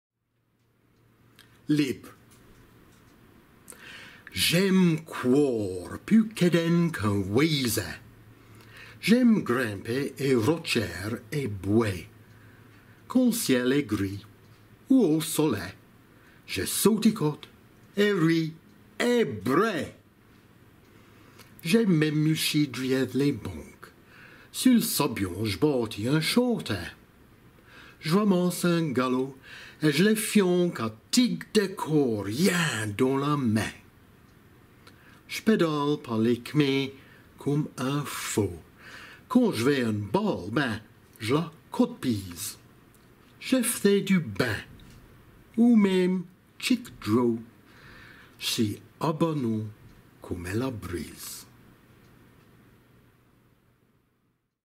Libre
J'aime couôrre pus quédaine qu'un ouaîsé;
j'aime grîmper ès rotchièrs et bouais;
quand l'ciel est gris ou au solé,
jé saûticote et ri et brai.

J'aime mé muchi driéthe les banques;
sus l'sablion j'bâtis un châté;
j'râmasse un galot et j'lé flianque,
à tigue dé corps, liain dans la mé.

J'pédale par les c'mîns coumme un fo;
quand j'vai eune balle, bein, j'la codpîse.
Jé f'thai du bein - ou même tchique dro:
j'sis à banon coumme est la brise.

Geraint Jennings